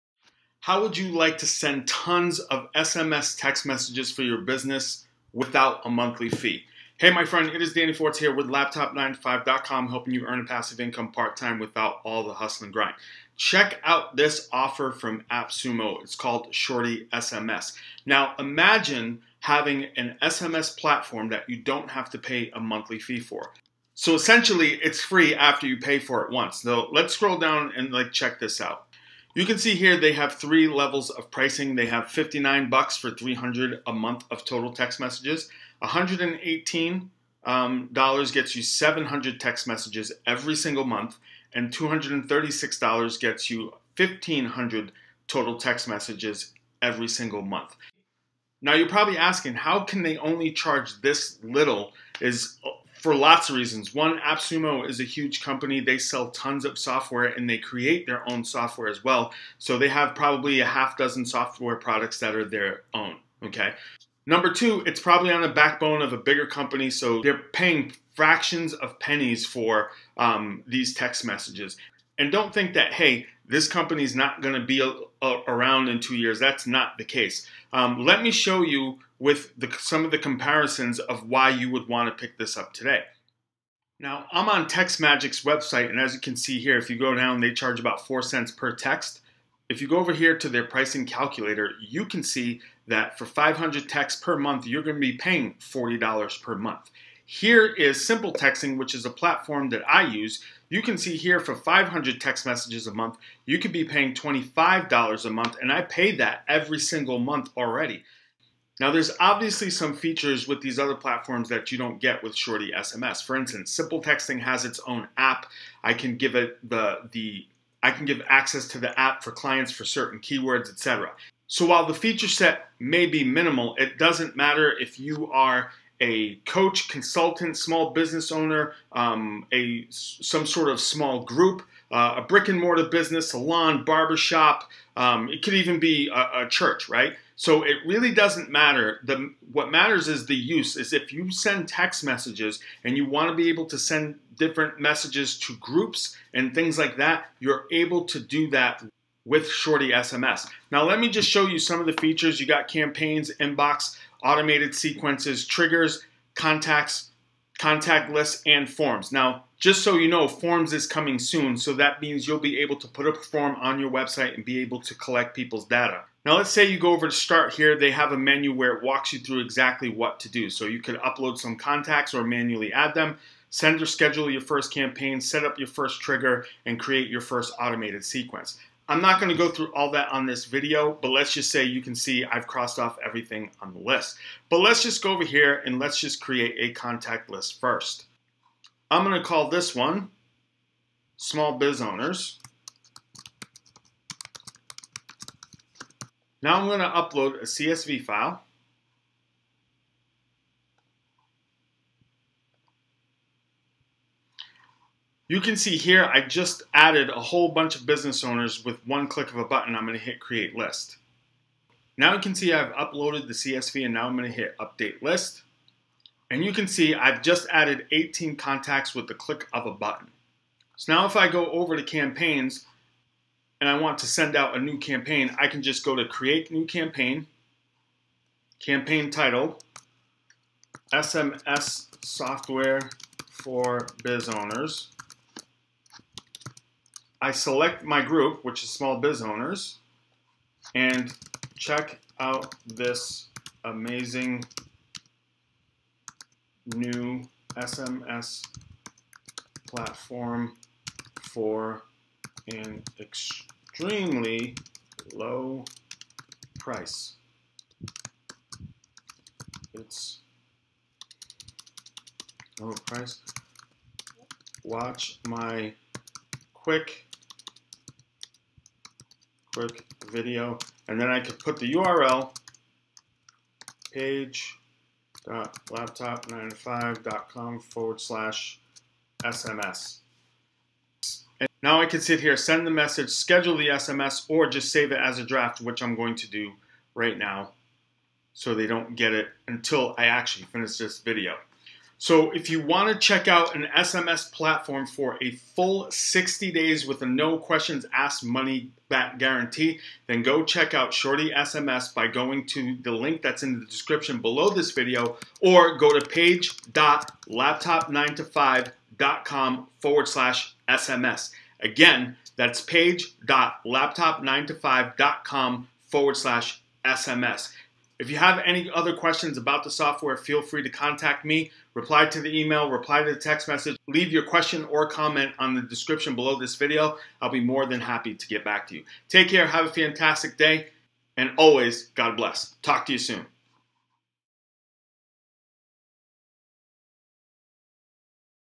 Shorty SMS Review App Sumo | SMS Text Messages with NO Monthly Fee
What is Shorty SMS and why should you pick it up? Shorty SMS is a text messaging platform created by App Sumo

Most SMS platforms have an ongoing recurring monthly fee. ShortySMS charges a one-time fee while you get access to all their features.

Here are some of the features of Shorty SMS -
-First you get your own private toll free number to send text messages from.
-Simple and easy-to-use dashboard to get started right away
-Create as many triggers/keywords you want with your account
-Create unlimited sms campaigns
-Built in lead generation forms to start collecting subscribers from your website (coming soon)
-Schedule time for SMS/text sends
-Automate entire sequences like an email campaign
-Automatically shorten and track URLs
-Clone campaigns with just one click

To get features like this you will be paying a minimum of $25 per month. With the deal that is on App Sumo you can get started with a one-time fee of just $59 for 300 sms sends each and every month.

Need more than 300 SMS credits in your account? No worries, they got you covered.

You can get 700 text messages per month for just $118 one time.
AND
You can get 1,500 text messages per month for only $236 one time.

Imagine what you can do to grow your business for yourself or managing campaigns for clients with Shorty SMS from App Sumo.